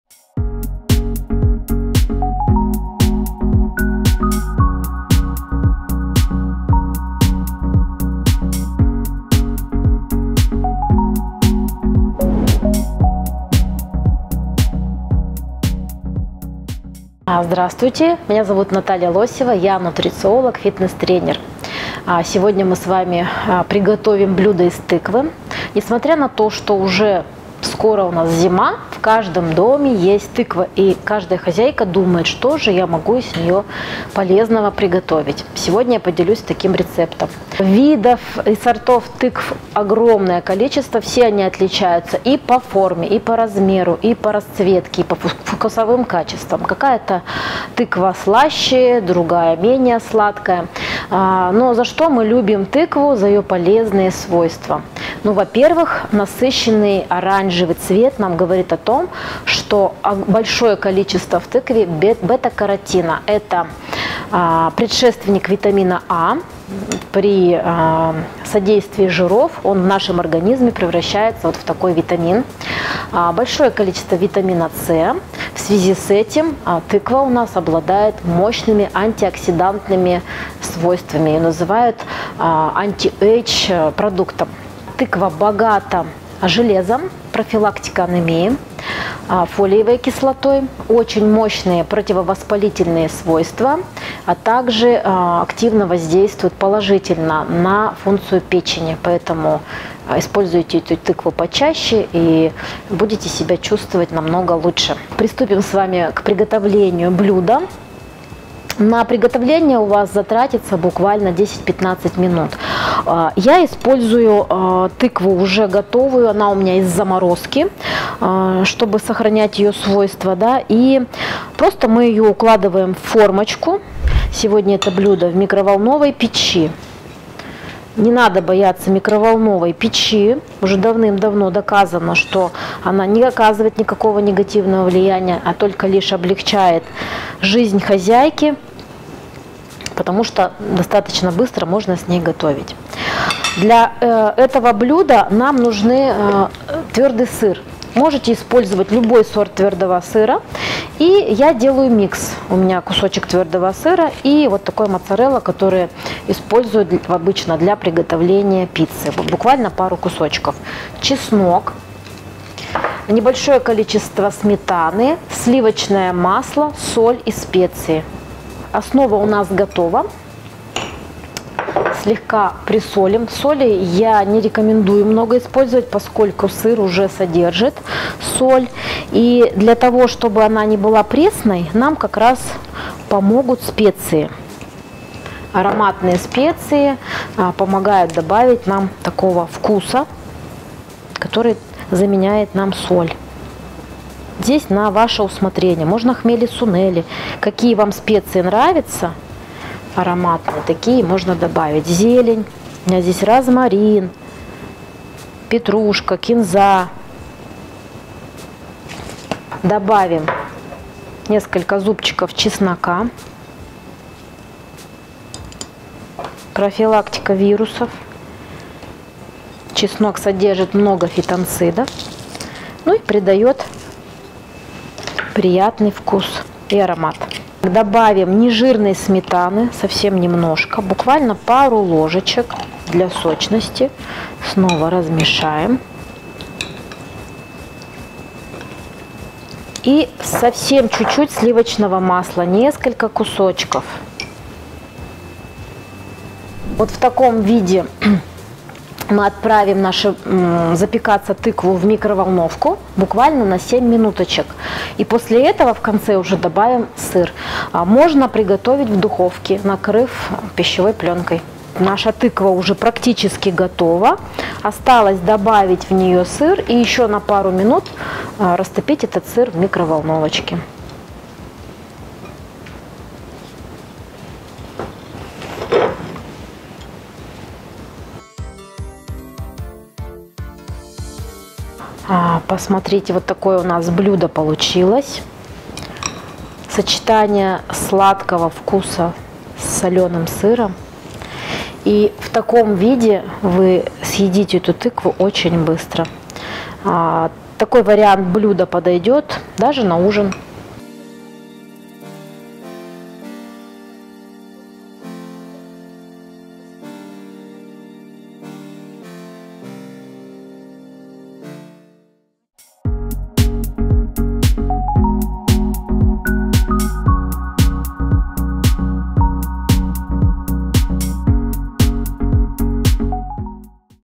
Тыква, запеченная со сметаной и сыром от Натальи Лосевой
Невероятно сочная, ароматная тыква, запеченная с чесноком и сыром порадует простотой приготовления. Аппетитное лакомство разнообразит домашнее меню и удивит родных. Это очень вкусно и полезно!